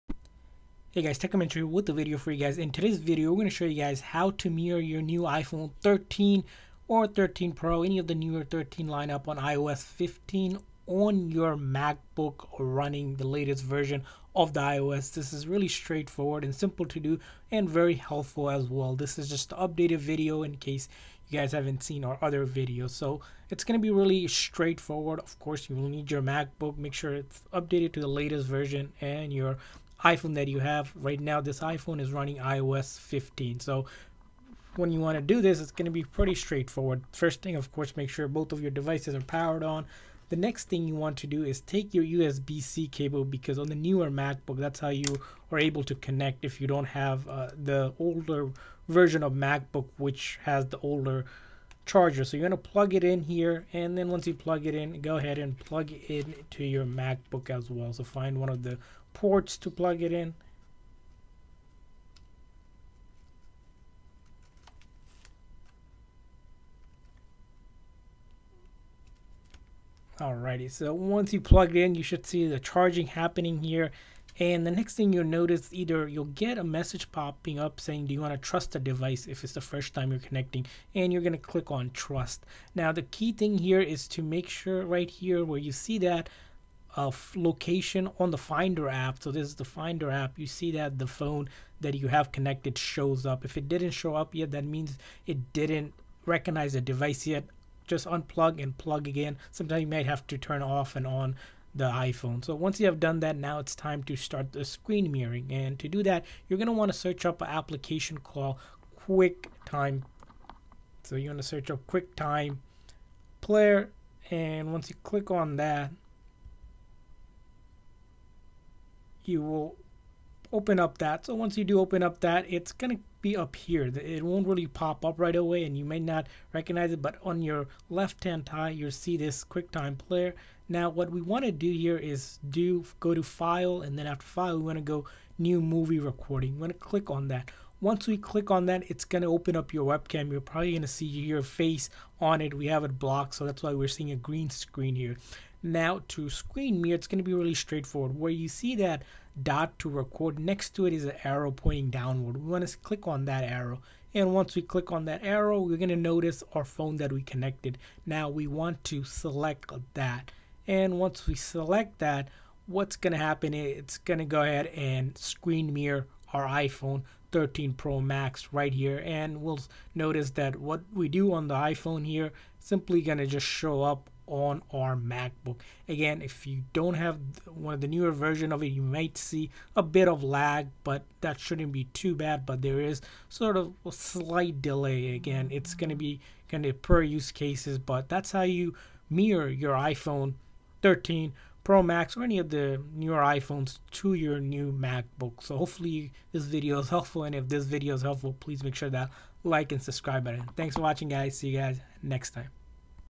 How to Screen Mirror iPhone to MacBook (2022)
We show you how to screen mirror / share your iPhone screen onto your MacBook. You can now project your iPhone screen onto your MacBook in real time. Everything you do on the iPhone will show up exactly on your MacBook screen as well.